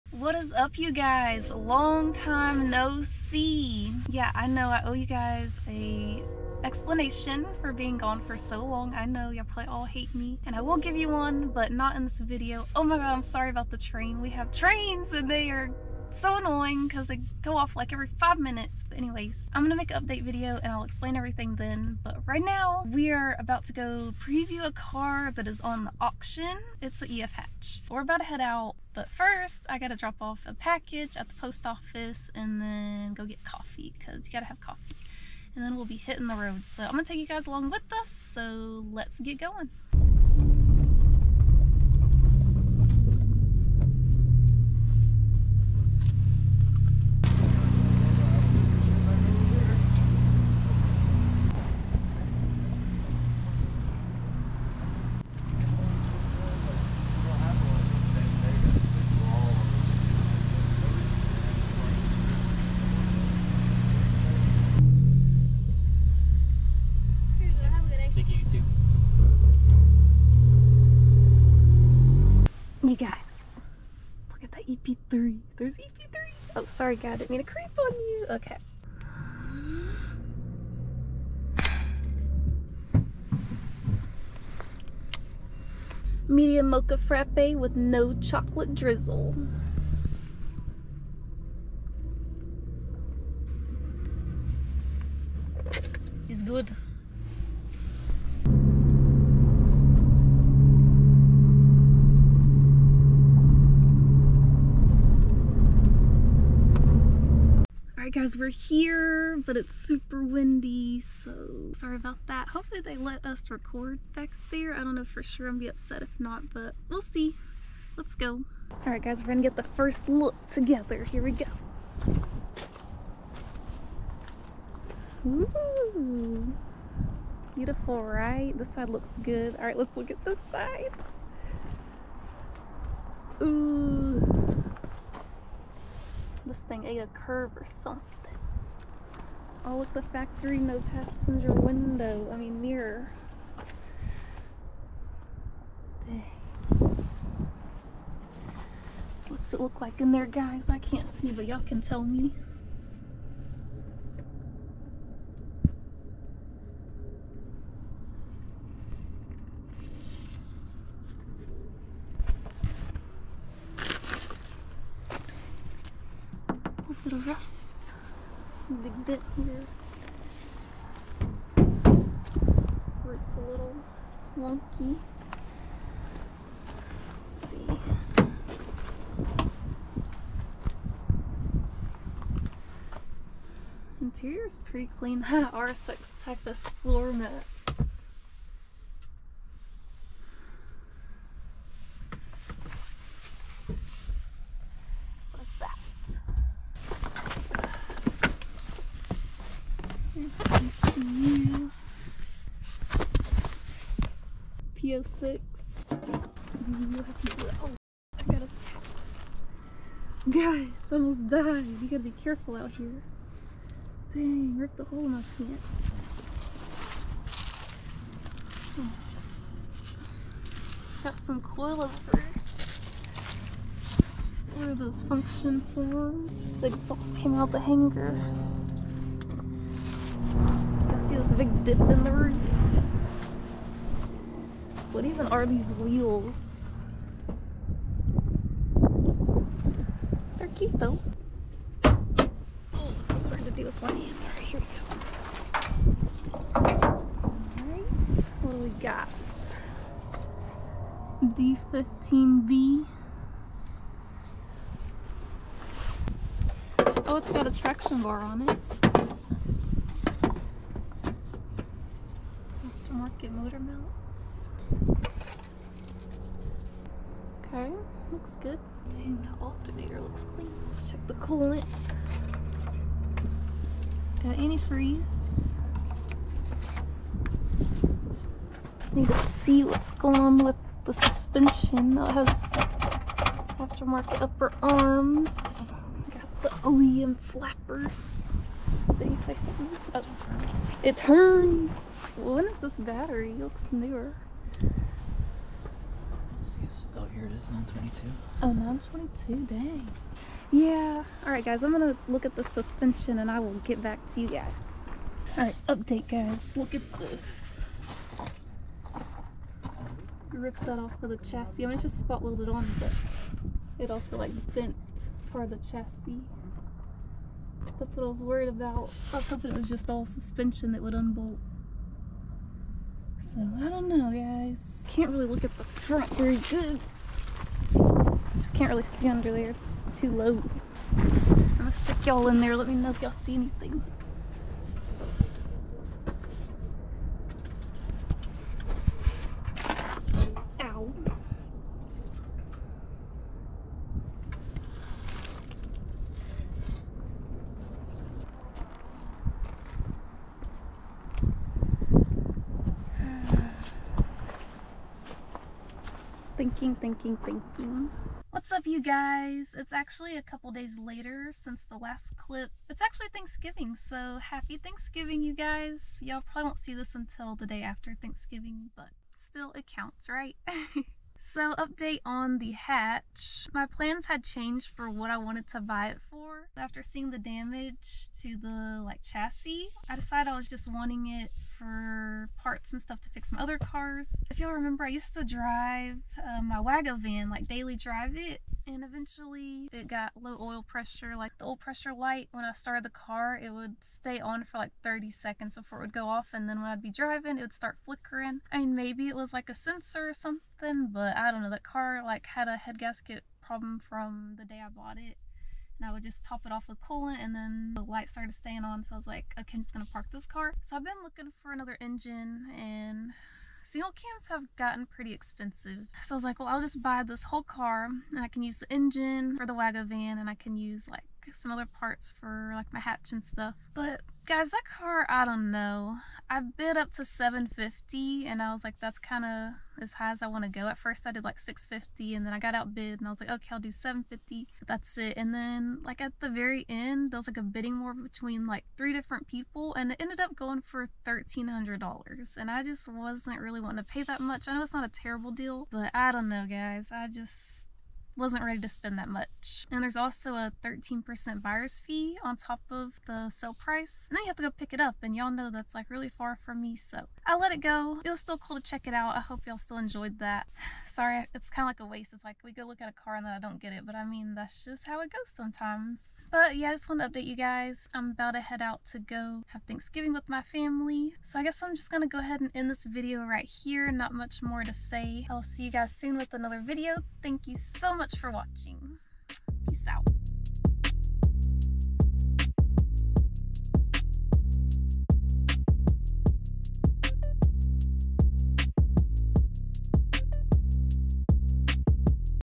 Previewing This Auction Hatch Reveals Hidden Damage
I thought I found a winter beater, but this damage changed my mind.